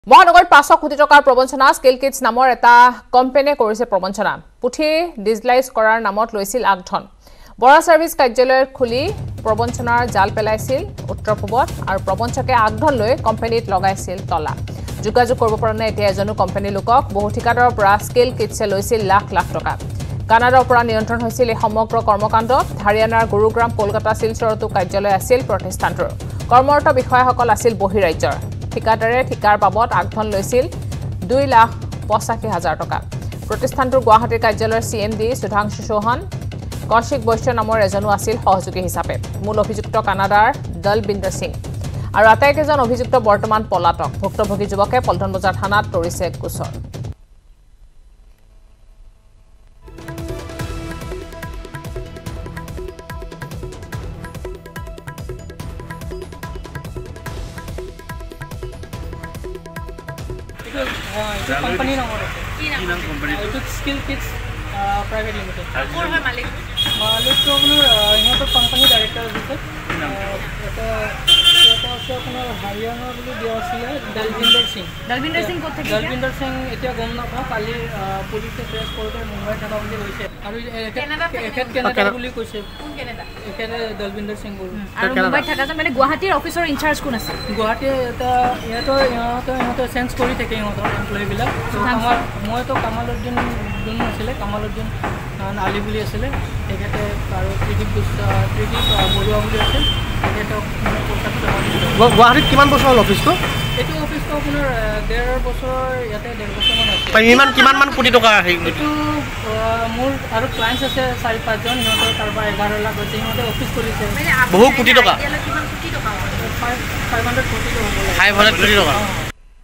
Guwahati News : ৫০০কোটি টকাৰ প্ৰৱঞ্চনা | Assamese News
Guwahati News | মহানগৰীত ৫০০ কোটি টকাৰ প্ৰৱঞ্চনা । প্ৰৱঞ্চকে আগধন লৈ কোম্পানীত লগালে তলা । n18oc_crime